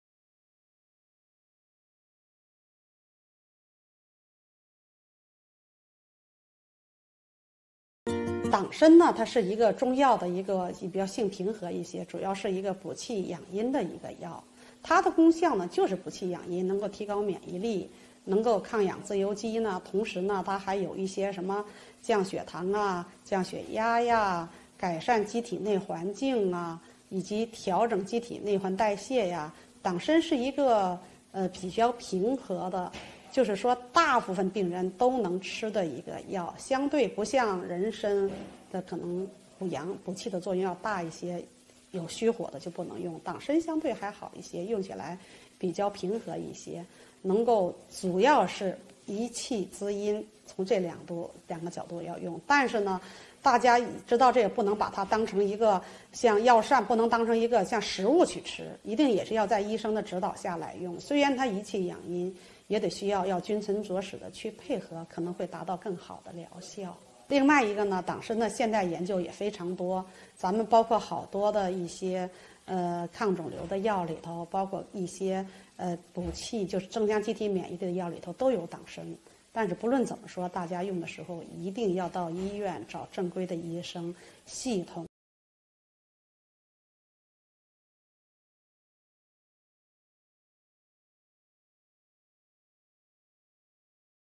党参的作用是什么 初玉平 首都医科大学附属北京朝阳医院
党参是中药的一个比较性平和的一个补气养阴的药物。它的功效就是补气养阴，能够提高免疫力，能够抗氧自由基。同时还可以降血糖，降血压，改善肌体内部环境以及调整肌体内部环境代谢。党参是大部分病人都能够吃的药物，相对于人参的补气作用要大一些。主要是益气滋阴从这两个角度用药，但是不能当成食物去吃，一定得再医生的指导下用。

What is the function of Dangshen
Dangshen is a kind of traditional Chinese medicine, which is relatively peaceful and nourishing qi and Yin. Its effect is to Nourish Qi and Yin, It can improve immunity, It can resist oxygen free radicals. It can also reduce blood sugar, Lowering blood pressure, Improve the internal environment of the body and adjust the metabolism of the internal environment. Dangshen is a drug that most patients can take, Compared with ginseng, it's more effective in Invigorating Qi. It is mainly from these two aspects, But not as food, It must be used under the guidance of the doctor.